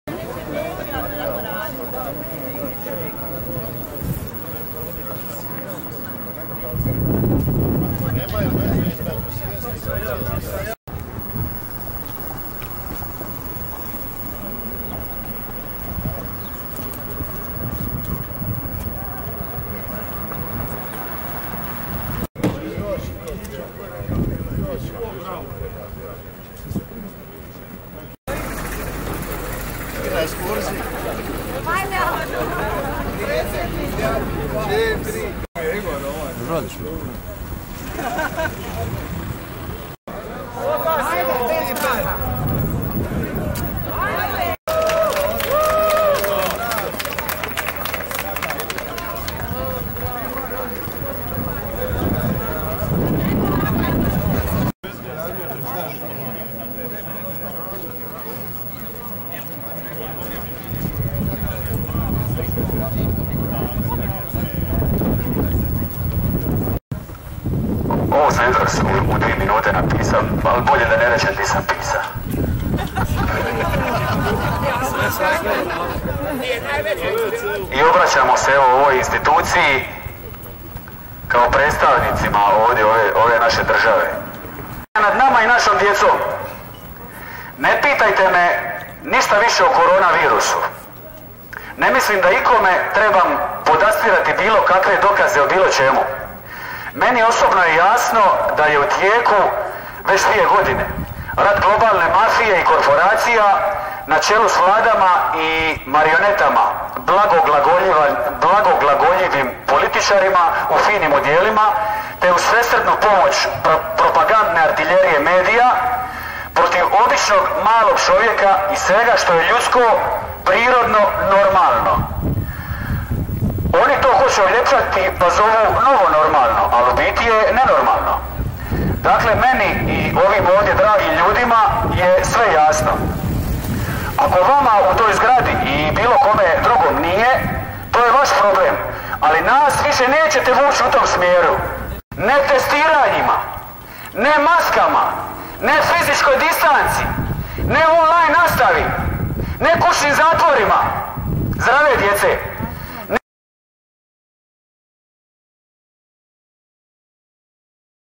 Ispred zgrade Županije u Šibeniku održan prosvjed protiv epidemioloških mjera
Prosvjed pod nazivom Mrš od naše djece održan je ispred zgrade županije u Šibeniku.